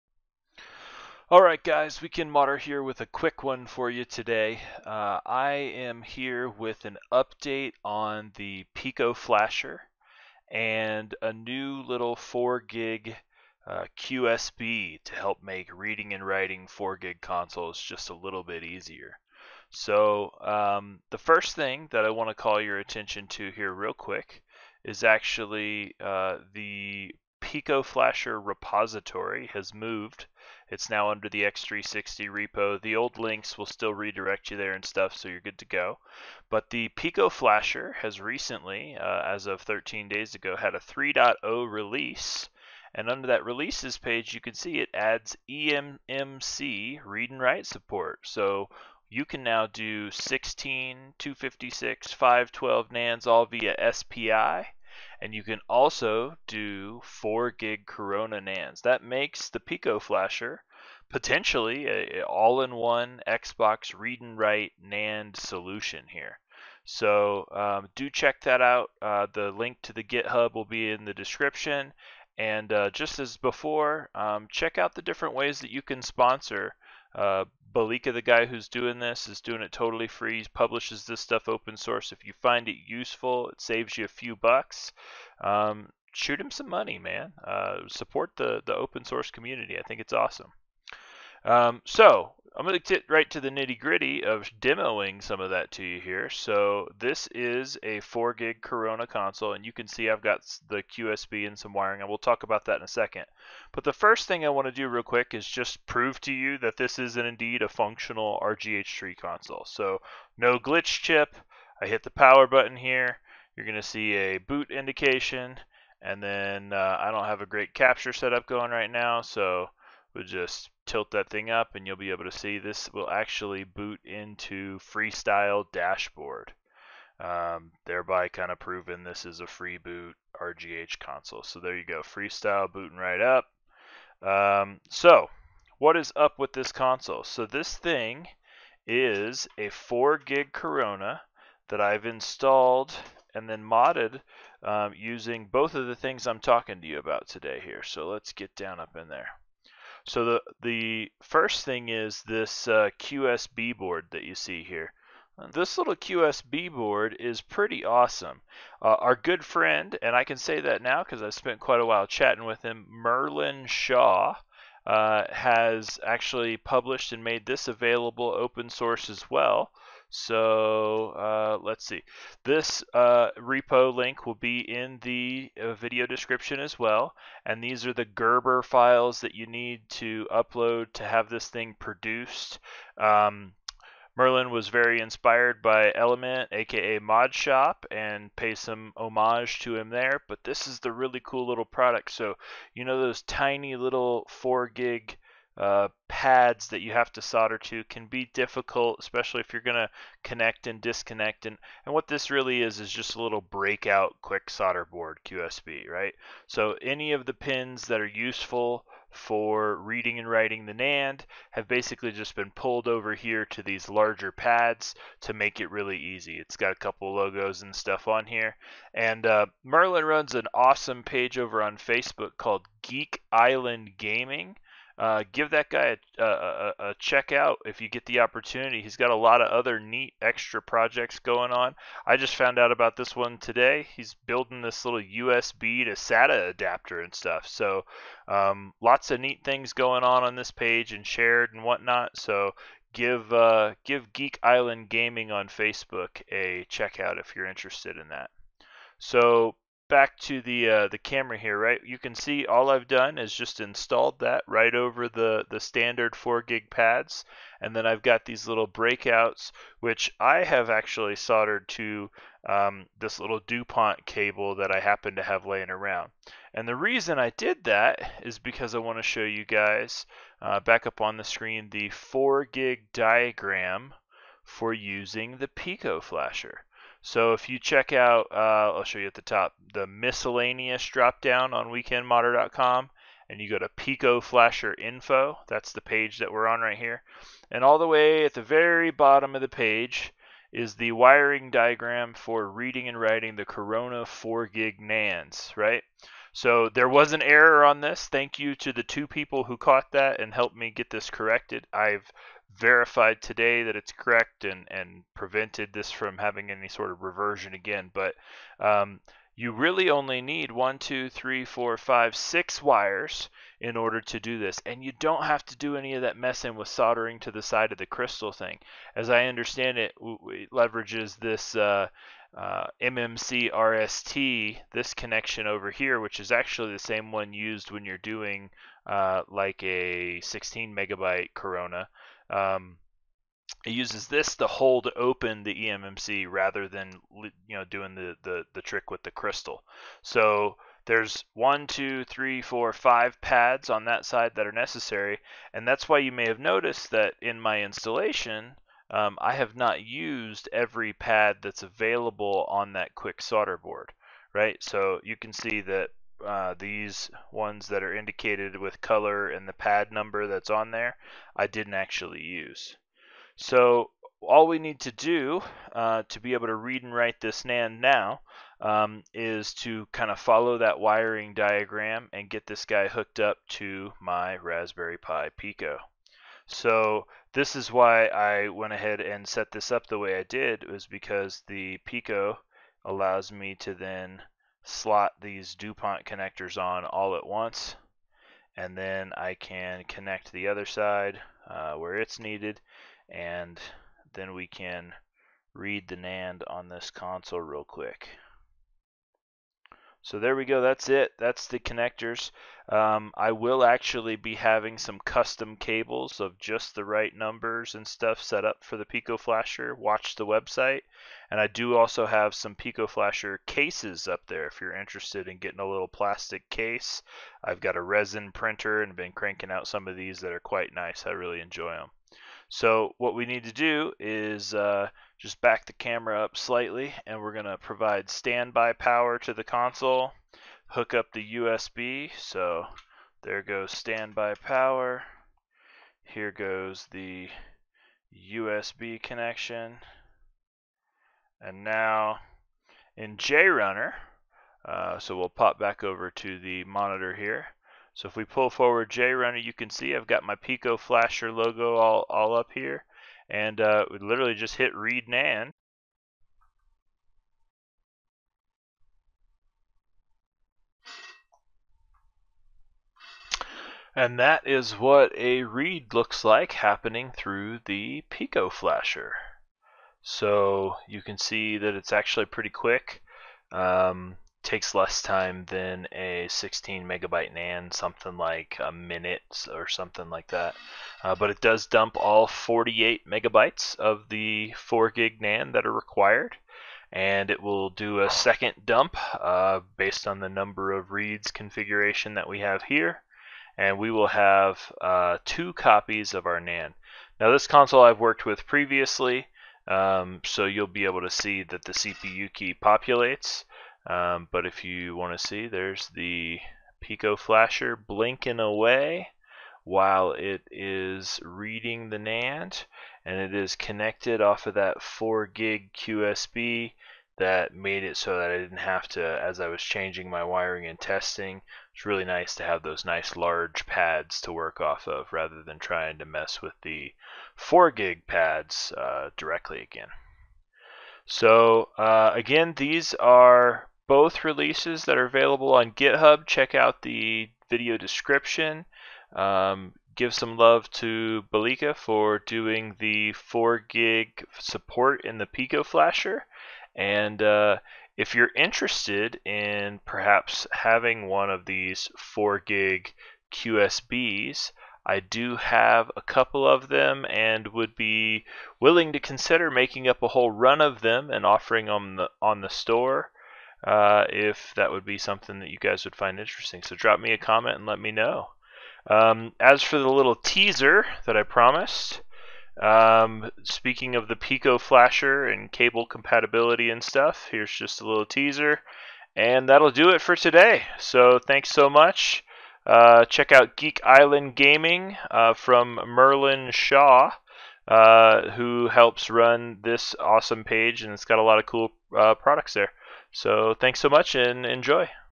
PicoFlasher 4gb Update and new 4gb QSB - plus a tease!
Picoflasher update release 3.0
-Fixes BB Phat reading
-Adds 4gb Reading support
-note, you need to use ZAdig for Win 7, 8.